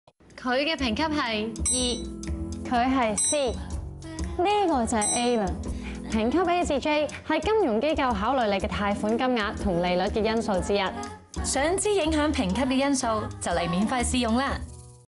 環聯 孫慧雪 唔吼靚仔吼宅男 廣告 [HD]
Transunion
環聯
我的信貸評級
環聯信貸評級
反映財務狀況
只需$280, 即可一個月內無限次查閱信貸報告及信貸提示服務